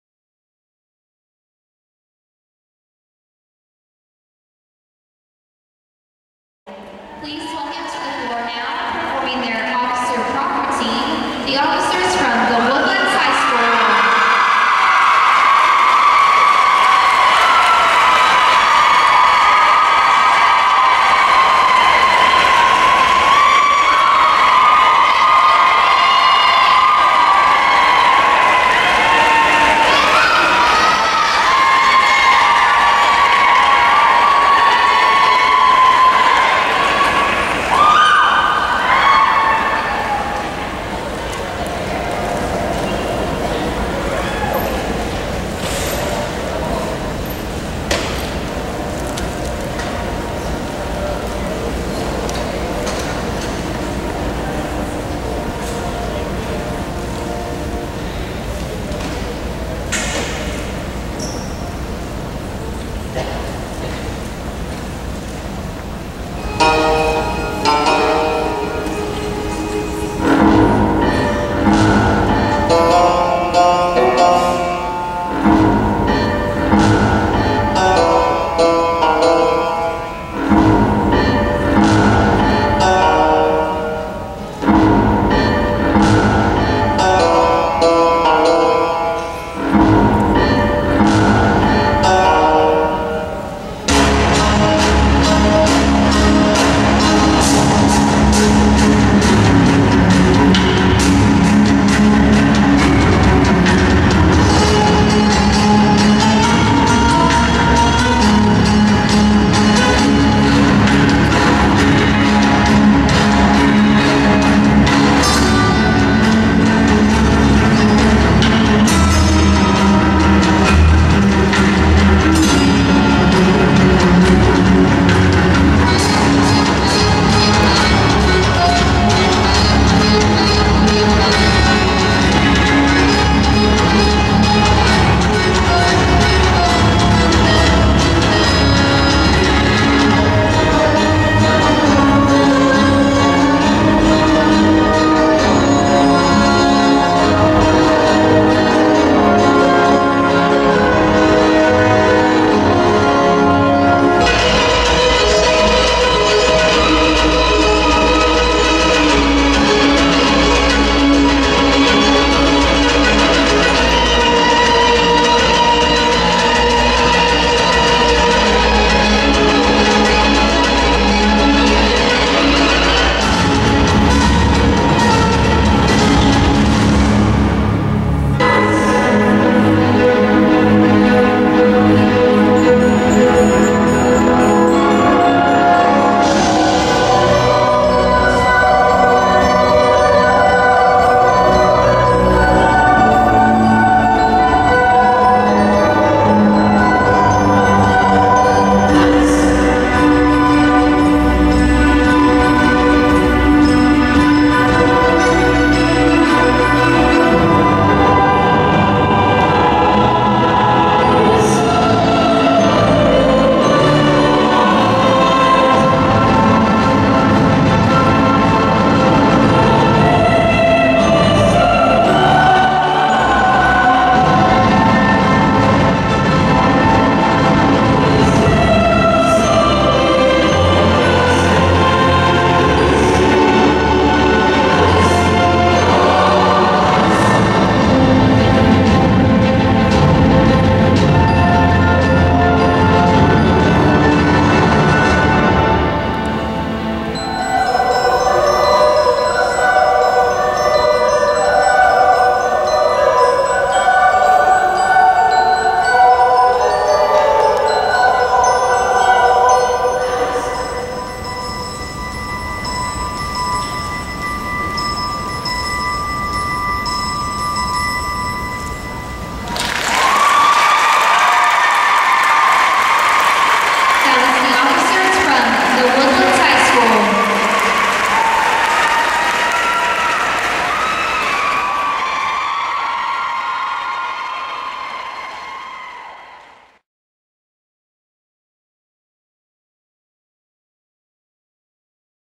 2011- 2012 TWHS Highsteppers - Officer Prop Routine 02182012
The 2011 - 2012 Woodlands High School Highstepper Officers perform their prop routine during the ShowMakers State Championship competition in San Marcos, Texas on Saturday, February 18, 2012.